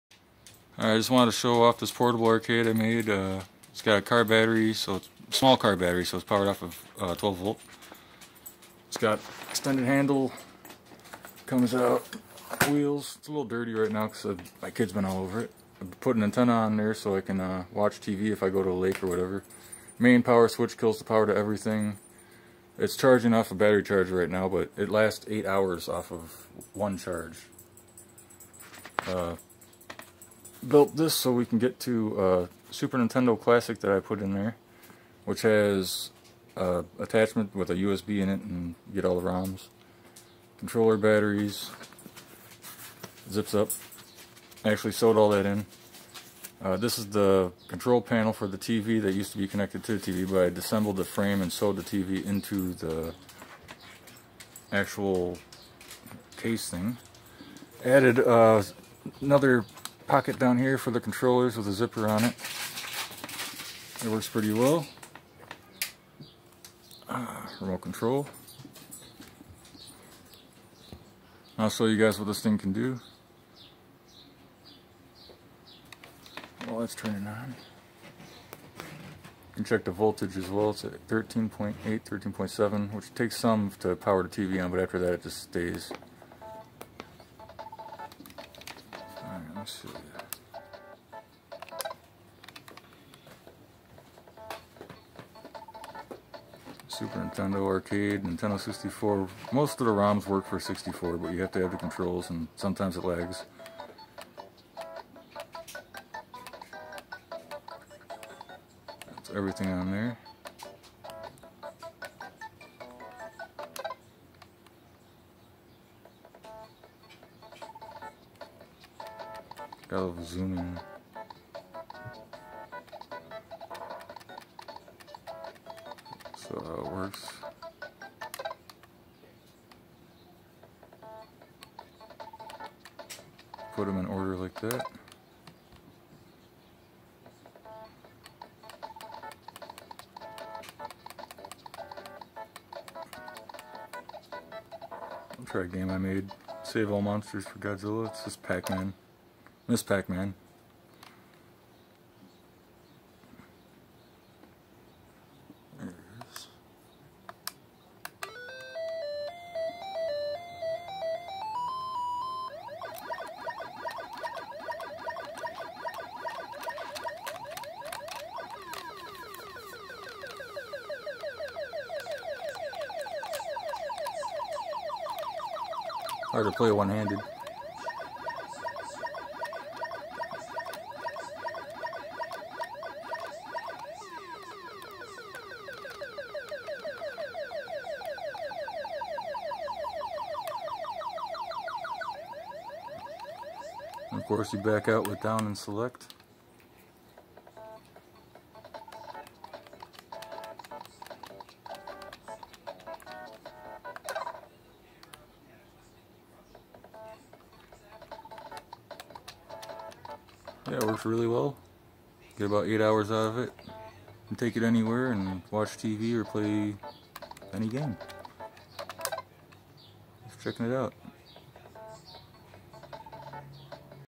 Travel bag arcade with SNES mini and c2m (classic to magic)
Built with a Jeep travel bag ' a SNES classic ' a c2m SNES cart transfer device with a USB port built in for putting roms on. Small ac to DC converter that stays cool.  TV powers the SNES and c2m ' using a small 12v car battery that's strapped in to prevent any movement ' added a pocket under the display for extra storage and mounted a PSP case (cut ' sew in ect) for heat release and so wireless controllers can make better connections ' also more storage space for the 2nd controller and extra batteries ' added a small HD antenna for TV channels which works well. All emulators work great but n64 can be spotty.